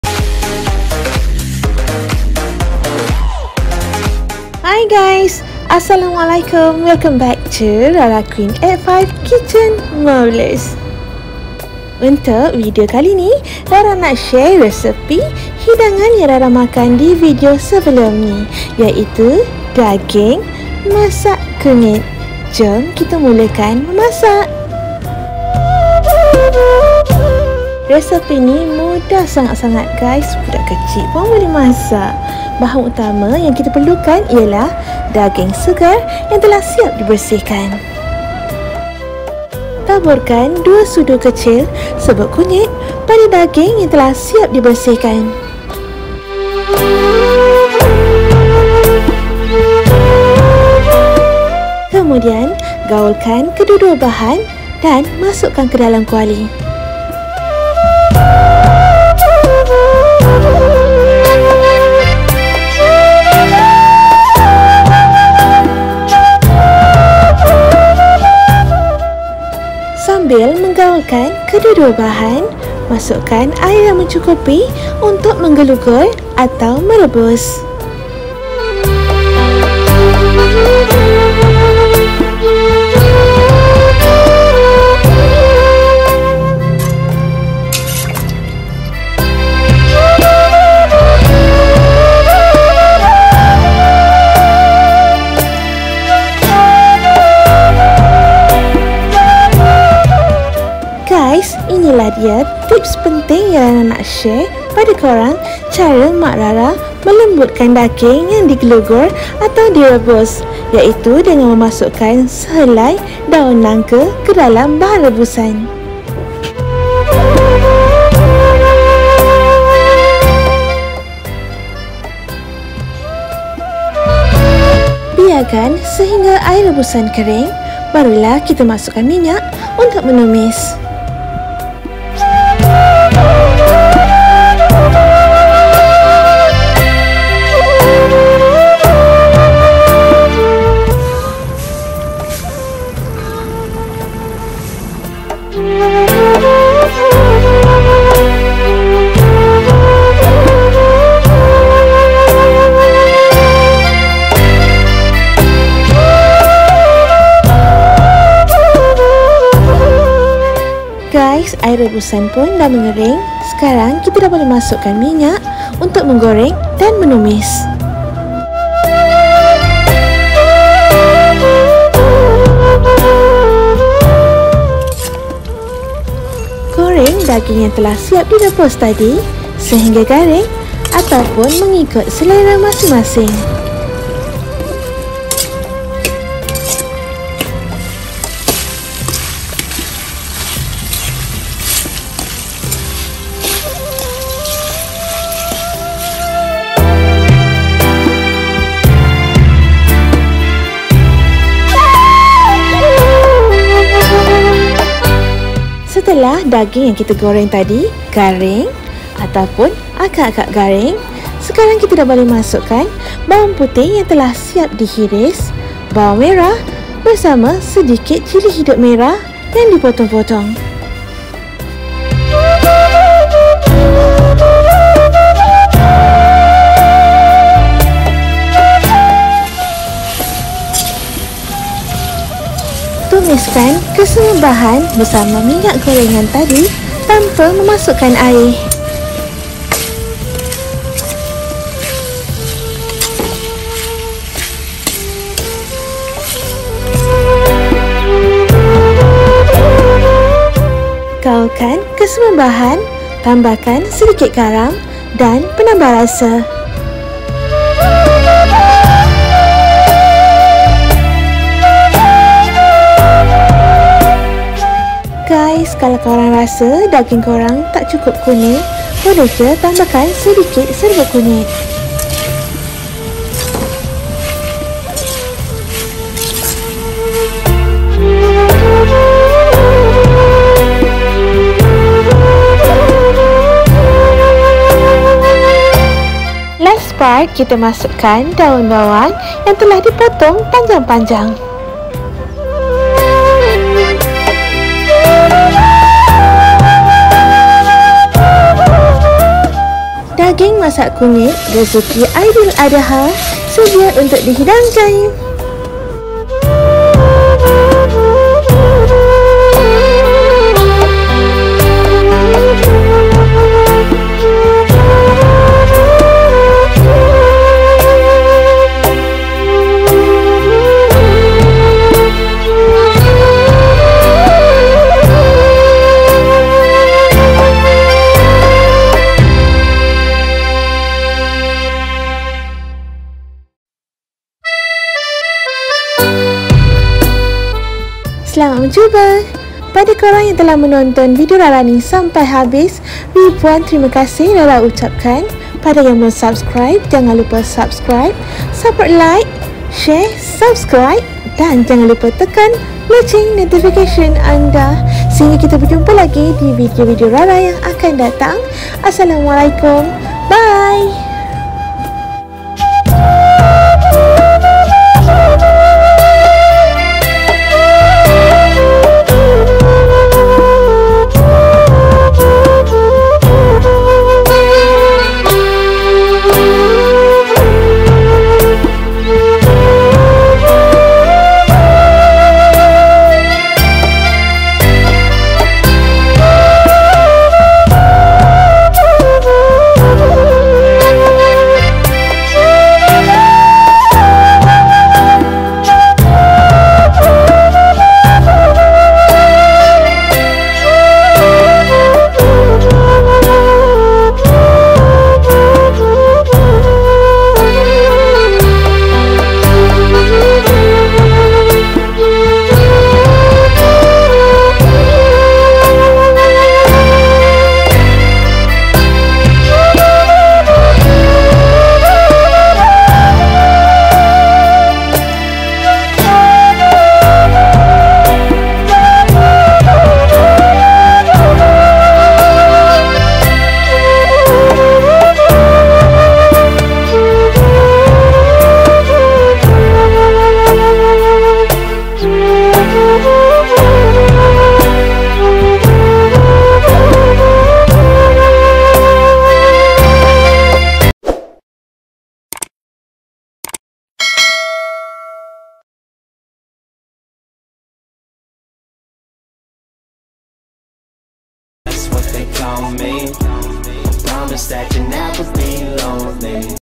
Resepi Daging Masak Kunyit & Tips Traditional Melembutkan Isi Daging Dgn Mudah
Resepi Daging Masak Kunyit

- Daging Segar

- Serbuk Kunyi  ( 2 @ 3 Sudu Kecil Bergantung Pada Daging Yg Di Masak )

- Air  ( Utk Menggelugur @ Merebus )

- Minyak

- Daun Nangka ( Sehelai )

- Cili Hidup ( Di Hiris )

- Bawang Merah Besar ( Di Potong Bulat - Bulat )

- Daun Bawang ( Di Potong Panjang )

Jgn lupa 🤗 Like! Tekan Loceng🔔 Notification anda & Komen yg manis manis ya🤗

Tonton sampai habis ok💪🏻😁

Follow ig rara @raraqueen85